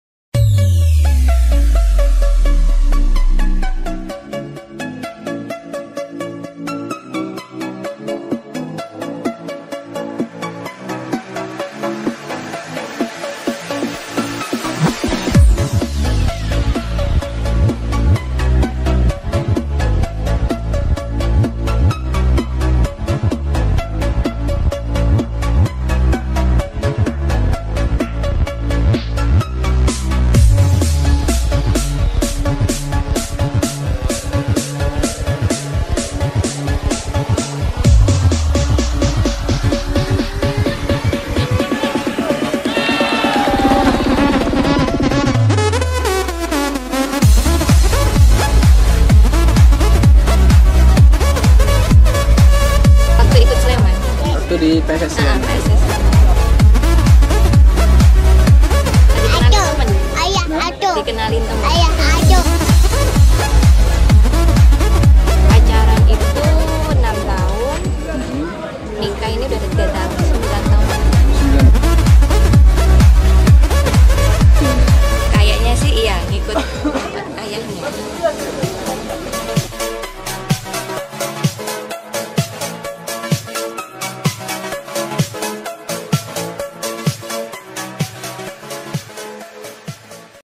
Cantiknya Istri Slamet Cahyo Saat Nonton Latihan
Istri dan buah hati Slamet Nur Cahyo ikut memberikan dukungan saat latihan di stadion Ratu Pamelingan Pamekasan Jawa Timur, Sabtu malam (18/02).